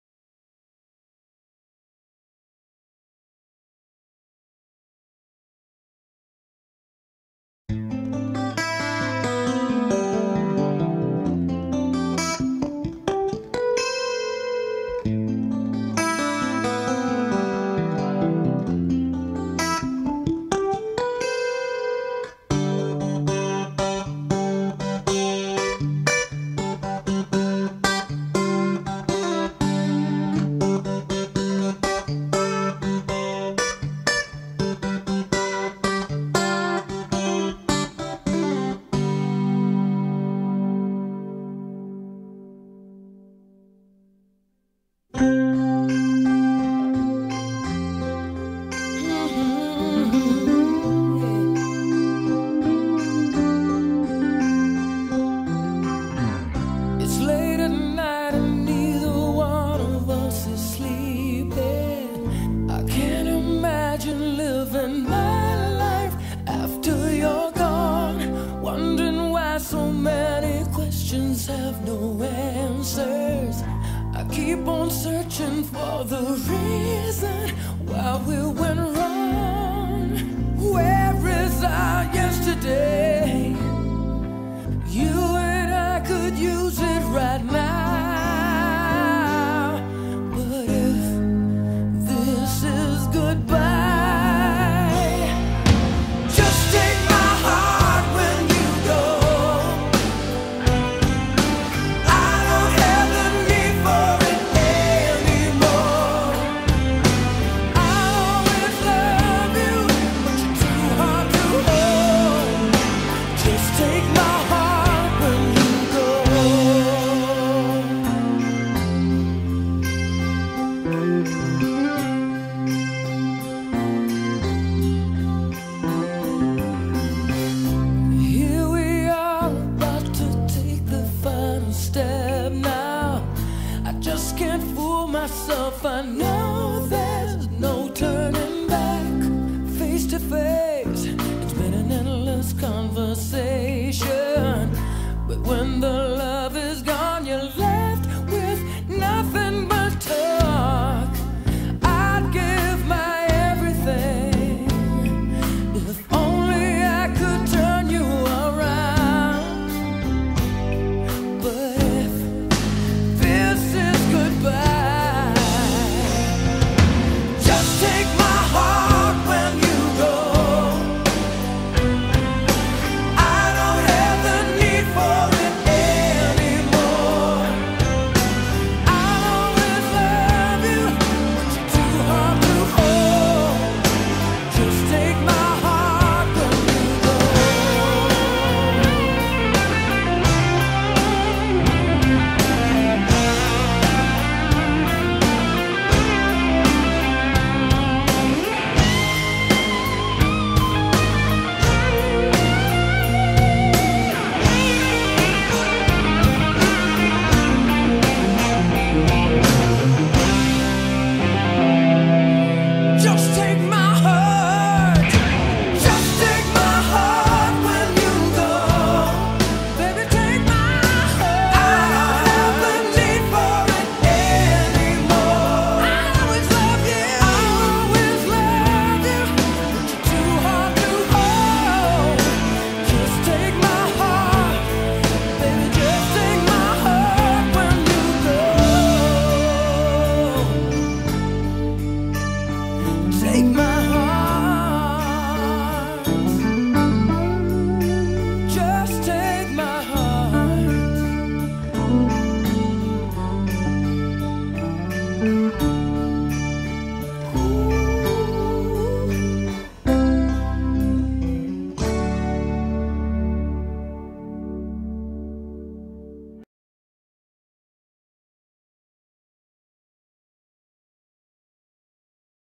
R.I.P. Pat Torpey
This is a video in honor of the late Mr. Big drummer Pat Torpey. Torpey died on Feb. 7, 2018.